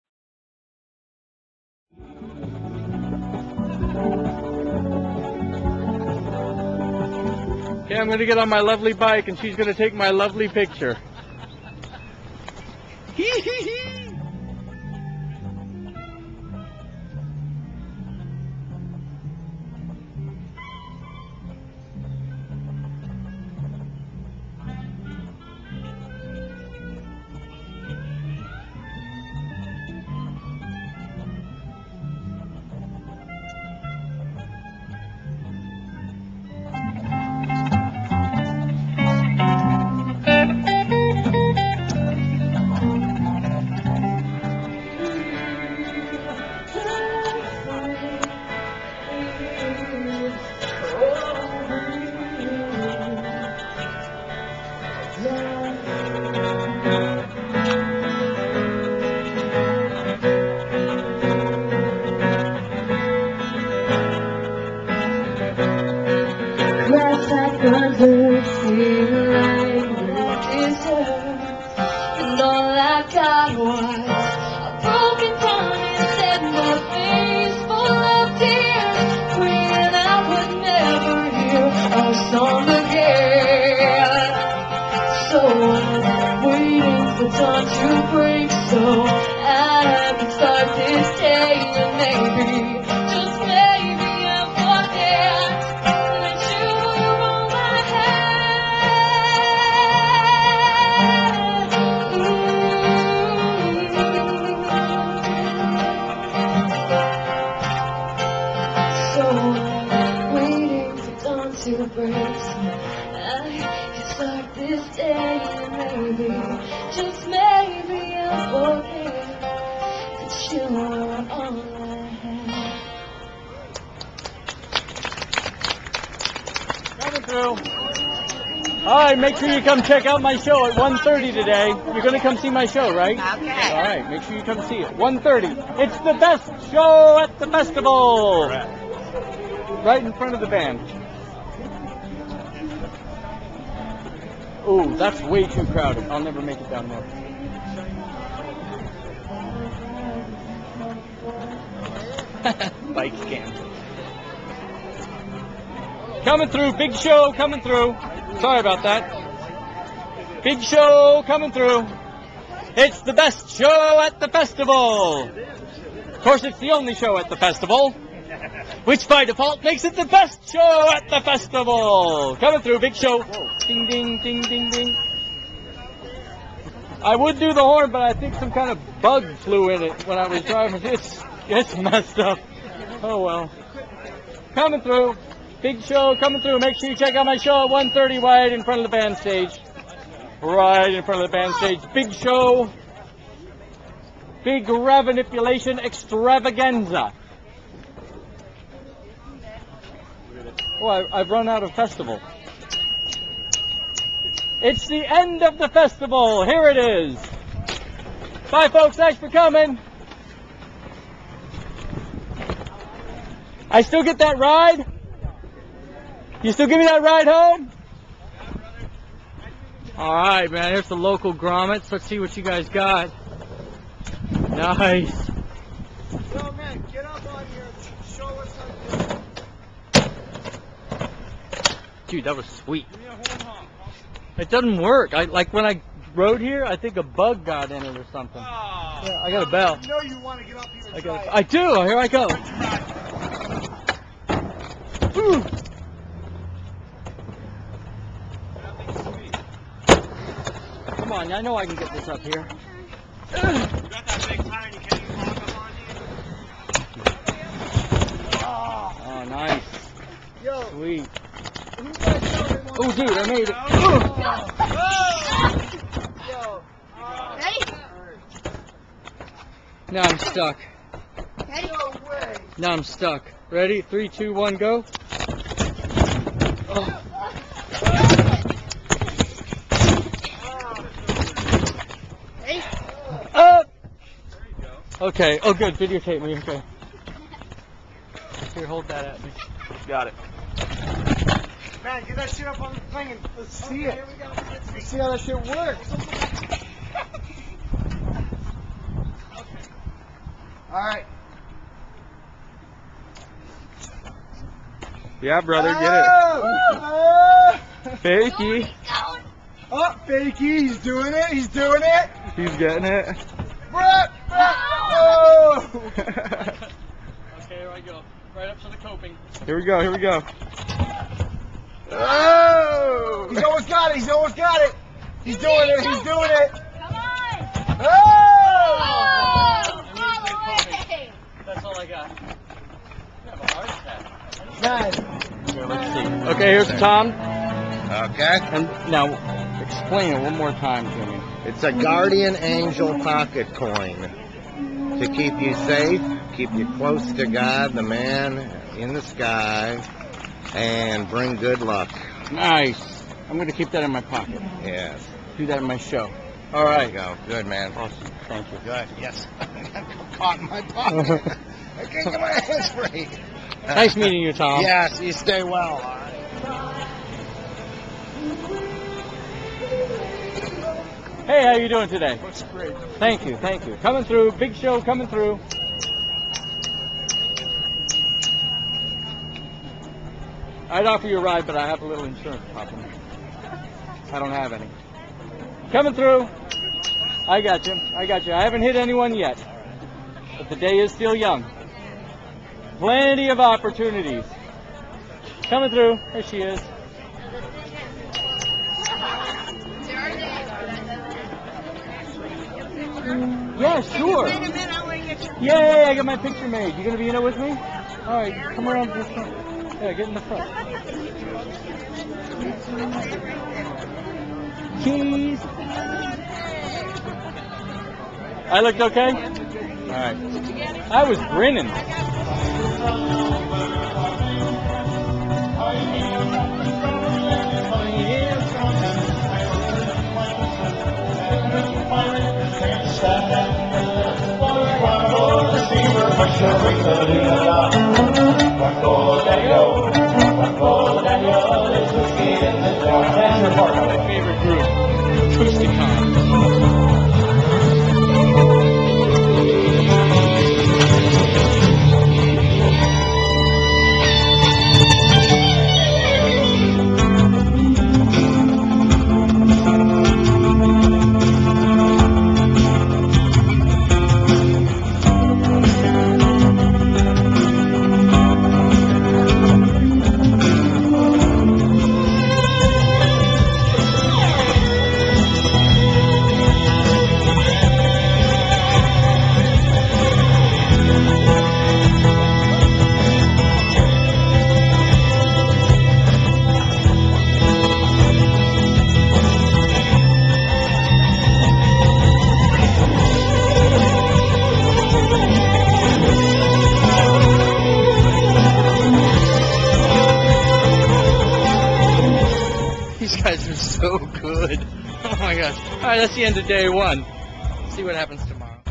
New Smyrna Beach Art Fiesta
Between shows at the Art Fiesta, New Smyrna Beach, Florida